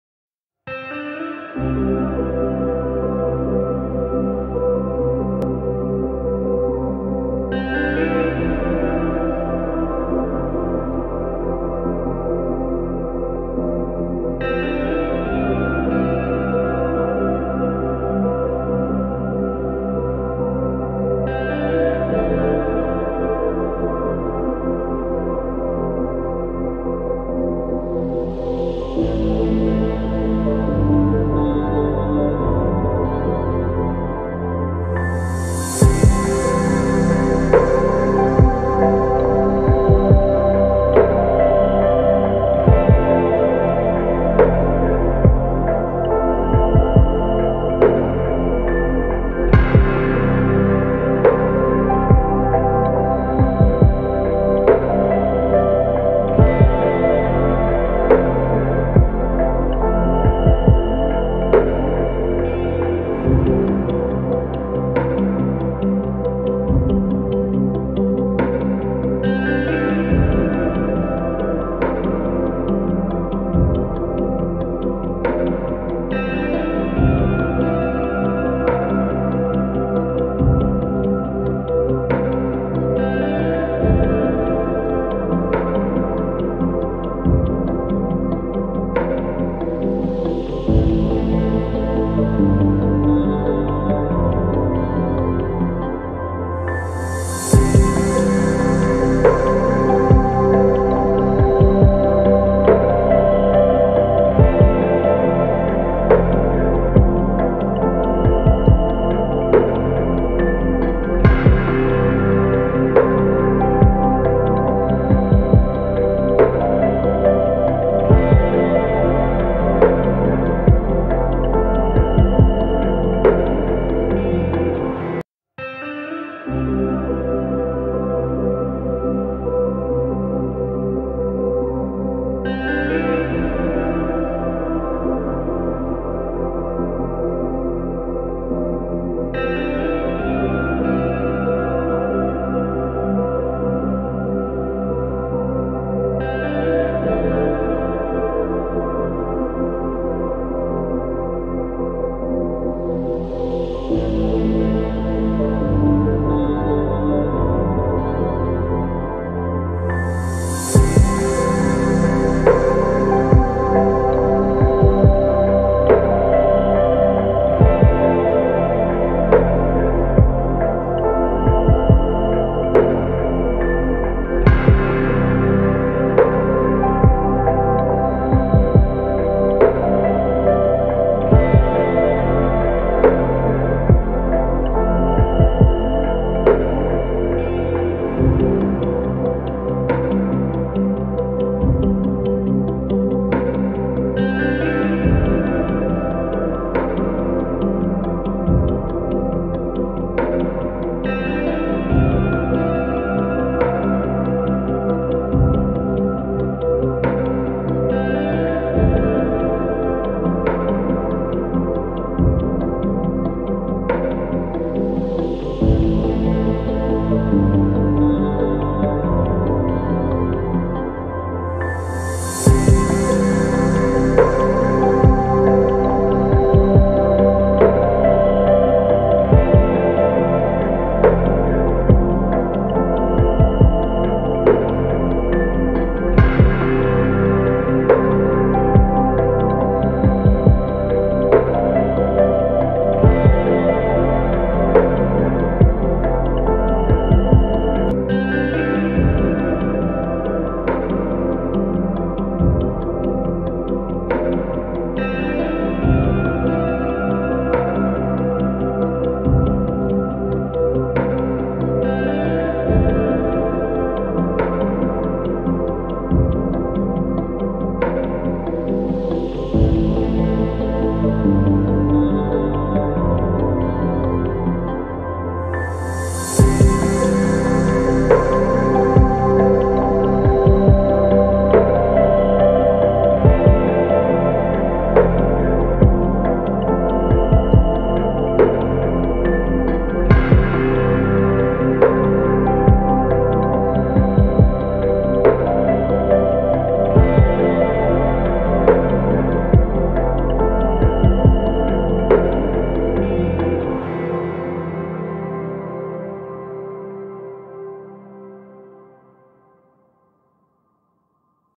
Drone view of forest with relaxing music | Meditation music | Music You Want
Here is the meditation music for relaxing your mind and body with the greenery of forests on the mountains. It's a really good relaxing music sleep and study also for concentration on anything you are doing.

You can find other relaxing music here;